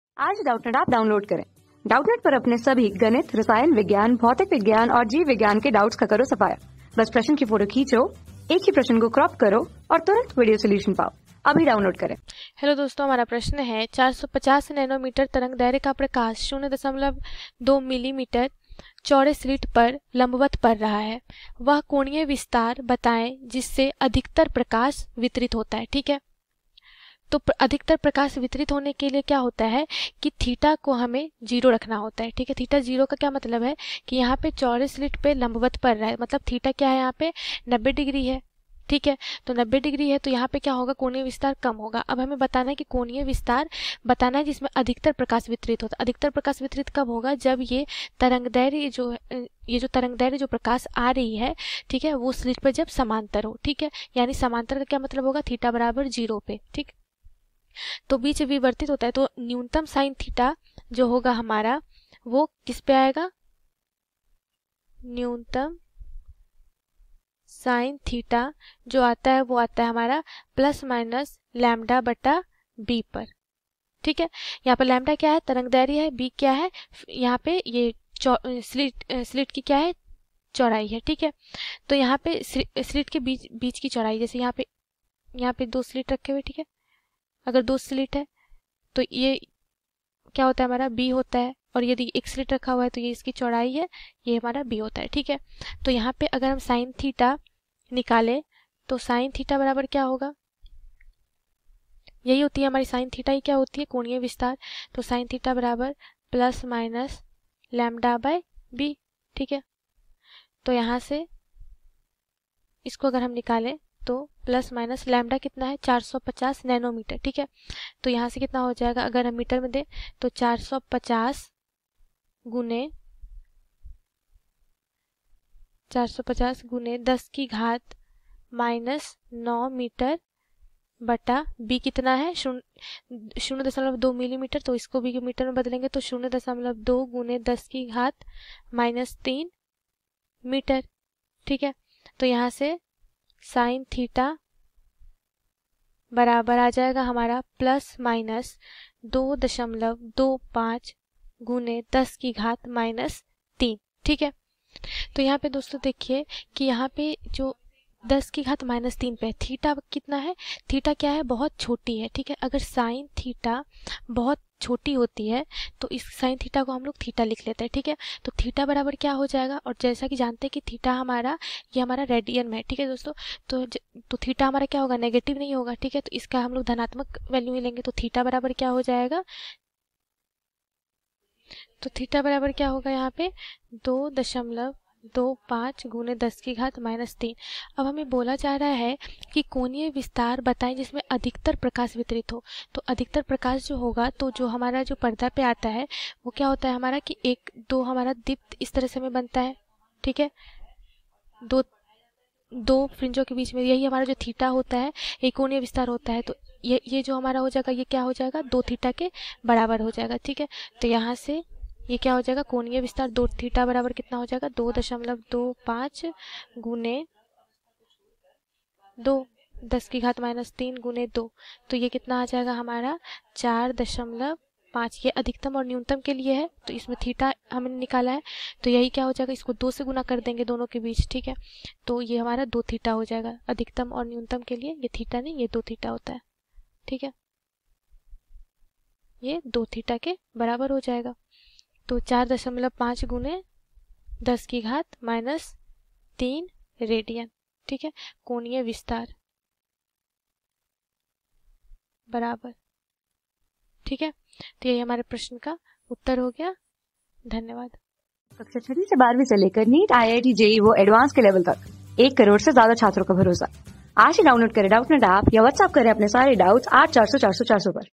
450 nm तरंगदैर्घ्य का प्रकाश 0.2 mm चौड़े स्लिट पर लम्बवत पड़ रहा है। वह कोणीय विस्तार बताएँ जिसमे...
450 nm तरंगदैर्घ्य का प्रकाश 0.2 mm चौड़े स्लिट पर लम्बवत पड़ रहा है। वह कोणीय विस्तार बताएँ जिसमे अधिकतर प्रकाश विवर्तित होता है।
Class: 12
Subject: PHYSICS
Chapter: प्रकाश का विवर्तन
Board:Bihar Board

👉 Have Any Query? Ask Us.